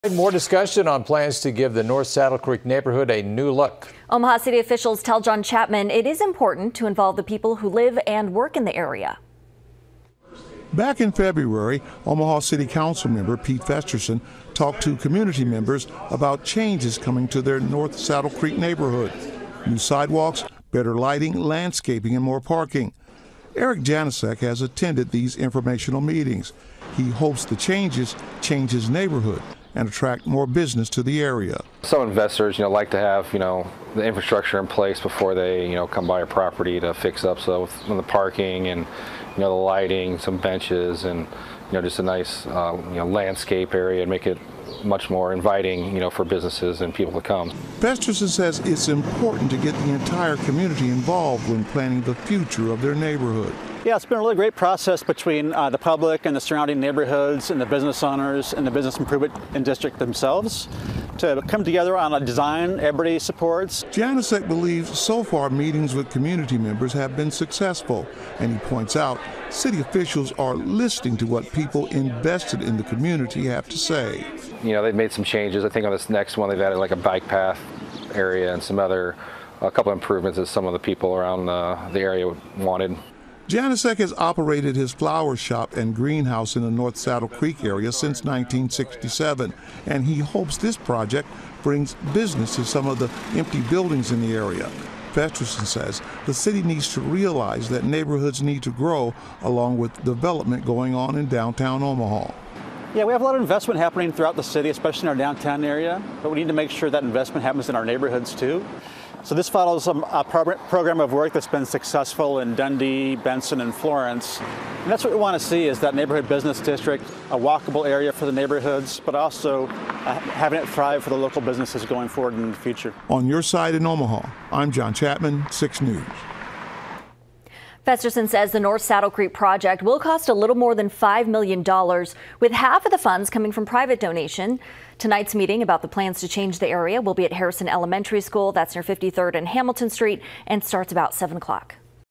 Omaha officials to host meeting on Saddle Creek development
Omaha officials are hosting a meeting Tuesday night to discuss the future of the Saddle Creek development project.